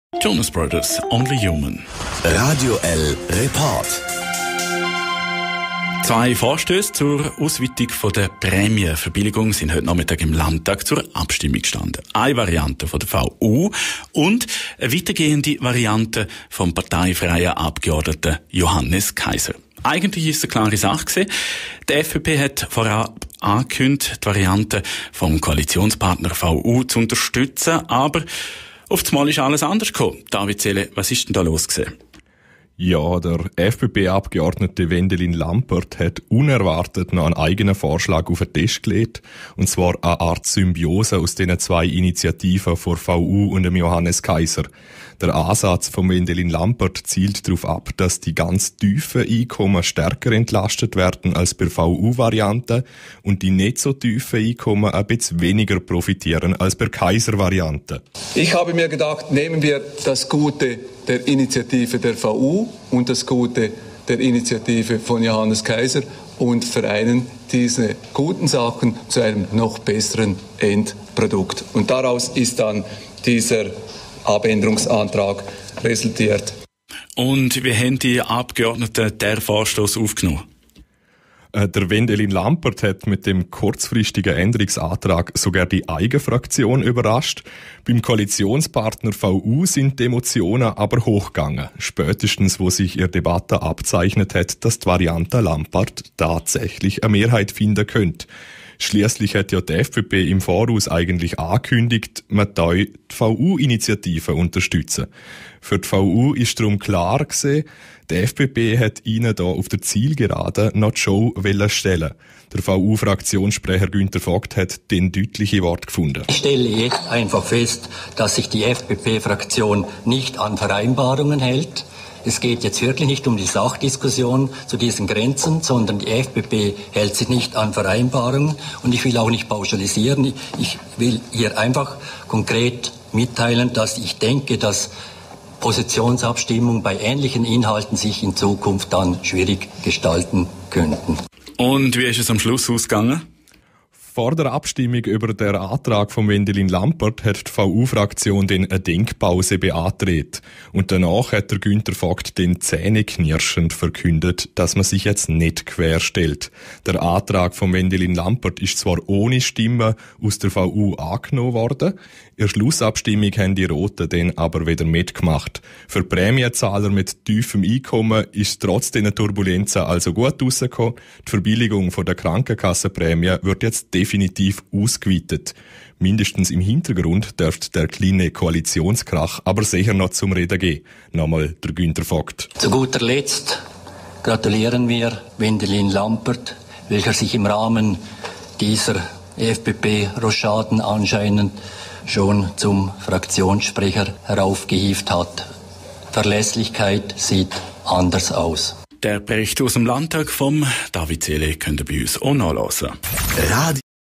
Koalitionskrach über Prämienverbilligung
Eigentlich ging es im Liechtensteiner Parlament um die Entlastung von Geringverdienern. Doch die Regierungsparteien VU und FBP sind sich dabei ordentlich in die Haare geraten.
(Bericht aus dem Landtag vom 7. November 2019)